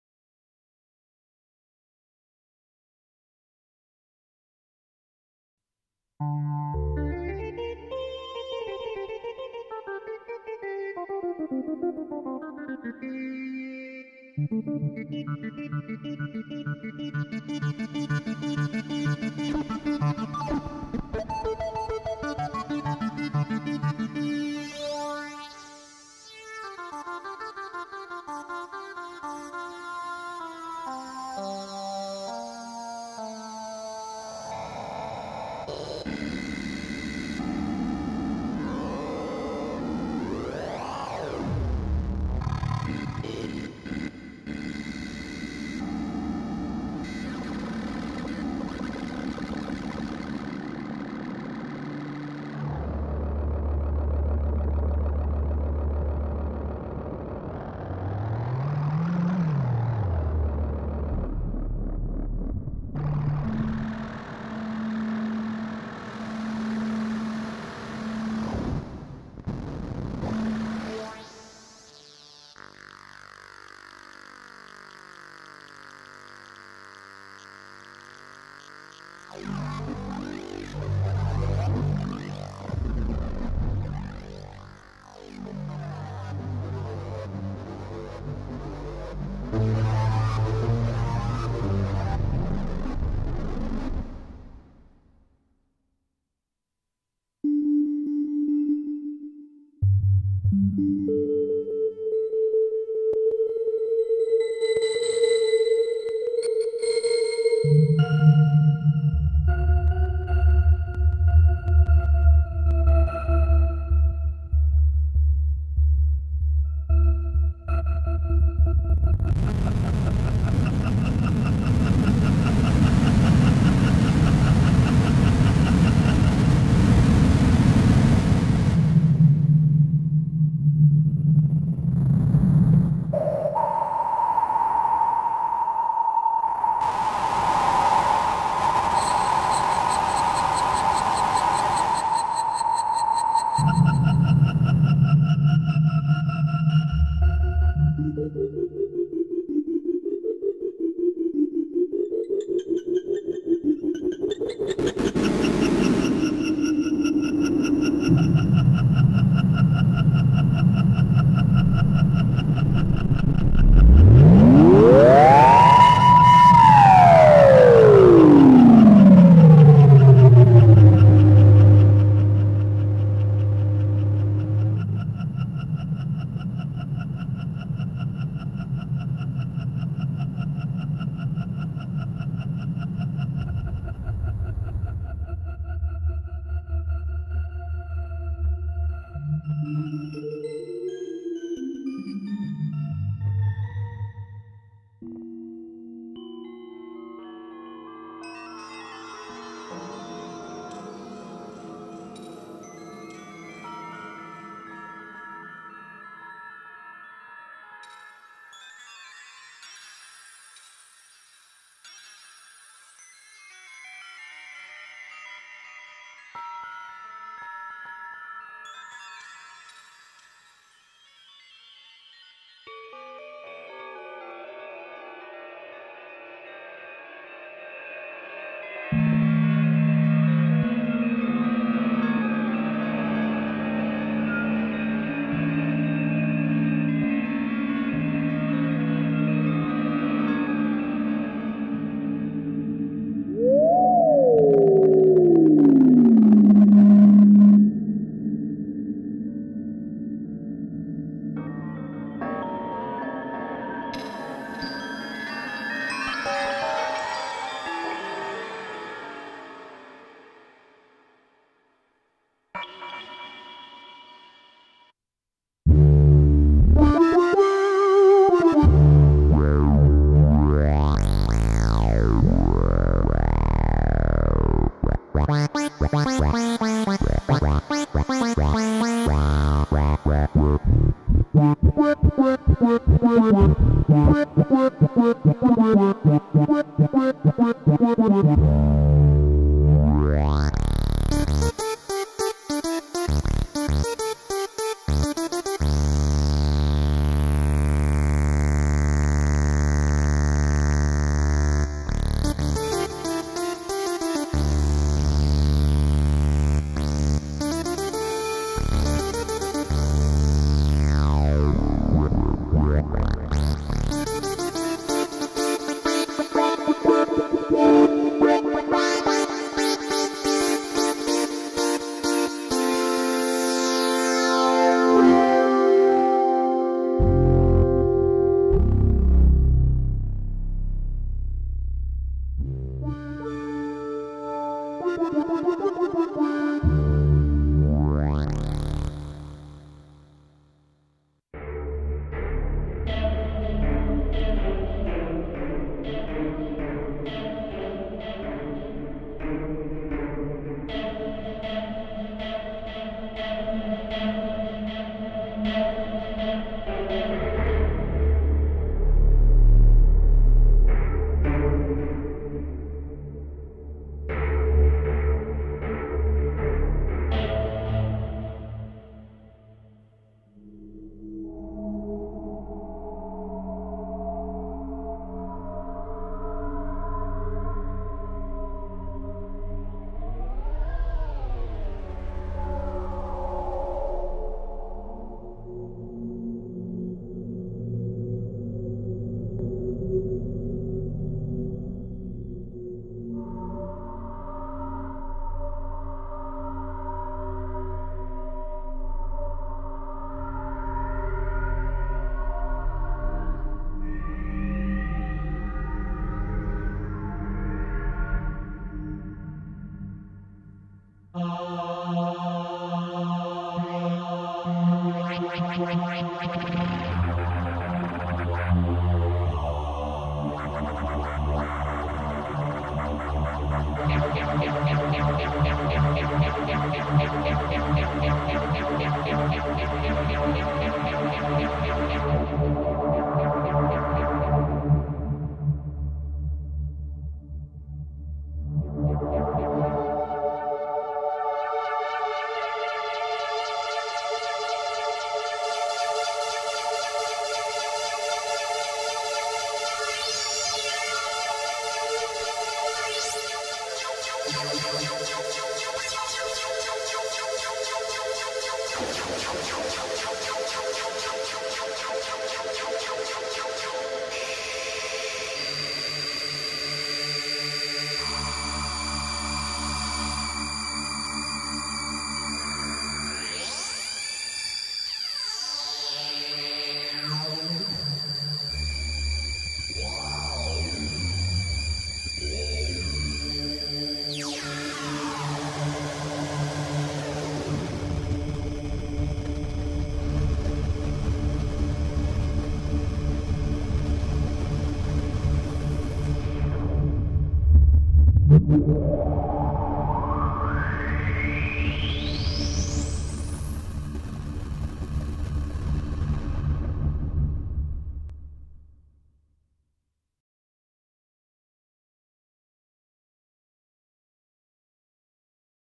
Waldorf Quantum NAMM Noodle - Just Music and Noises
The frequencies in this video change quickly. Be careful if you're using headphones. The Waldorf Quantum sounds amazing. But it's so deep you can hardly do justice to it in an 8 minute video, especially when you're just going blind and there is a line behind you waiting to get on the thing. But here's 8 minutes or so of just sound. And it was a true experimentation, not knowing really how this thing works so keep that in mind. Lots of soundscape stuff here since everyone knows what your "everyday" synths sound like.